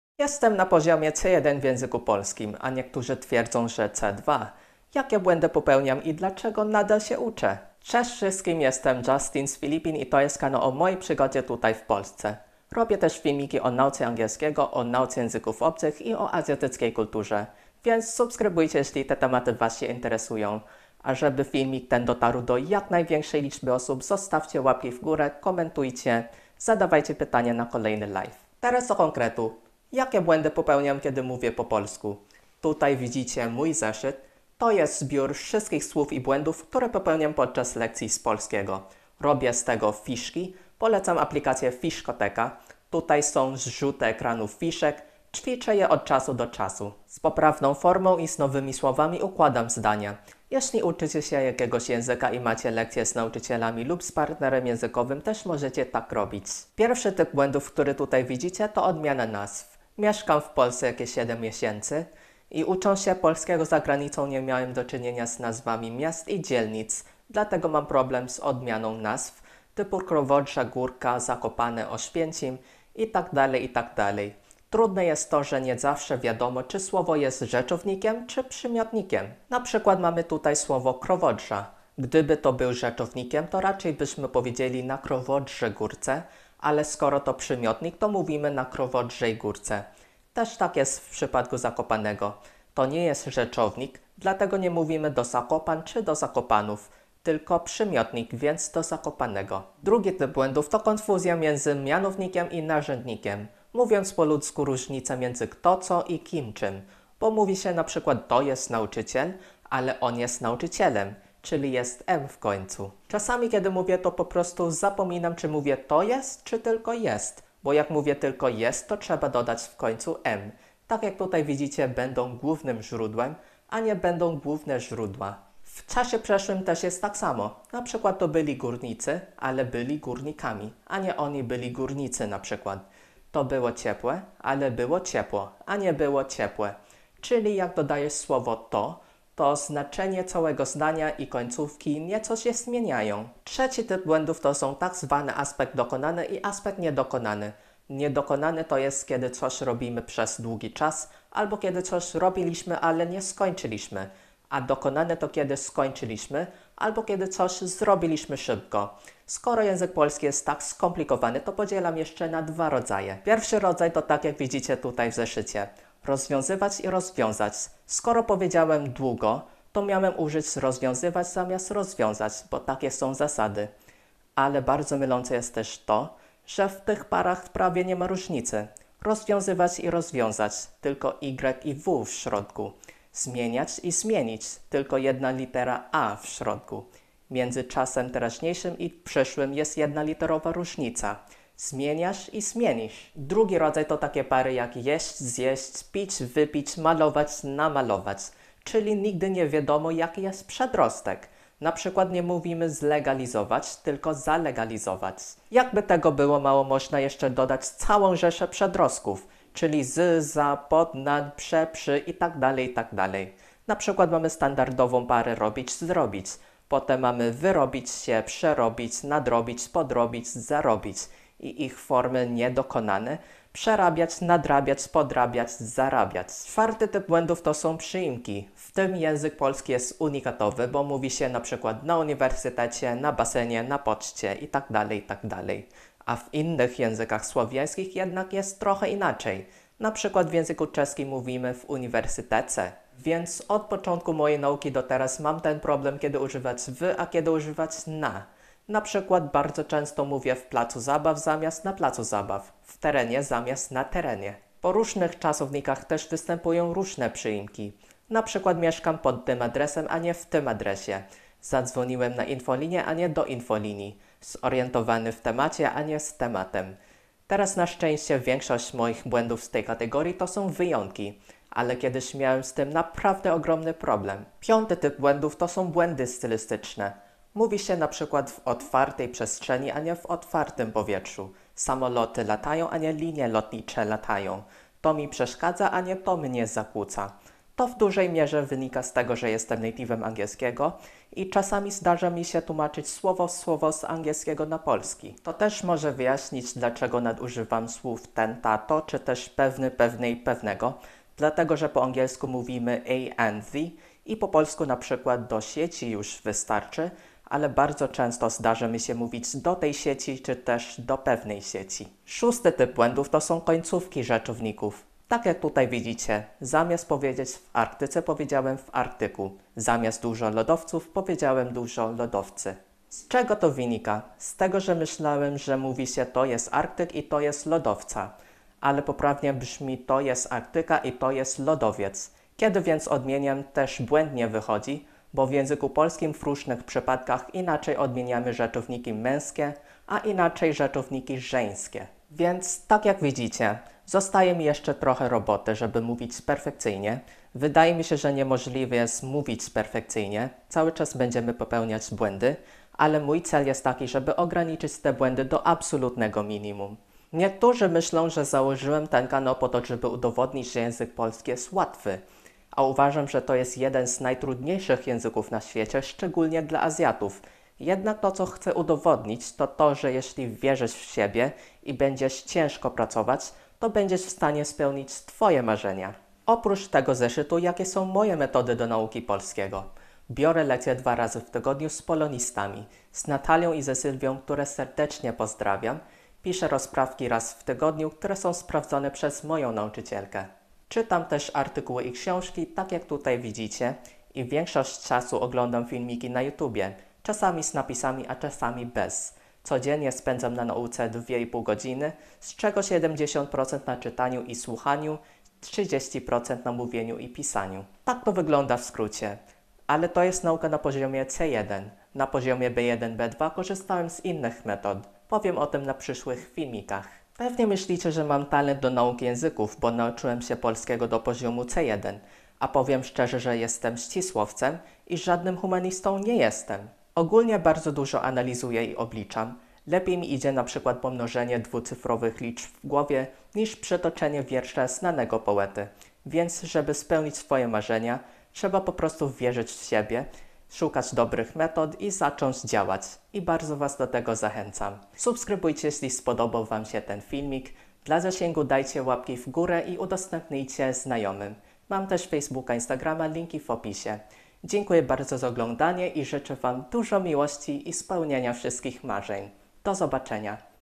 Cudzoziemiec mówi po polsku! Poziom C1 - jakie błędy popełniam i dlaczego nadal się uczę?!
Cudzoziemiec mówi po polsku! Dzisiaj filmik o tym, jakie błędy popełniam, czyli dlaczego język polski jest trudny!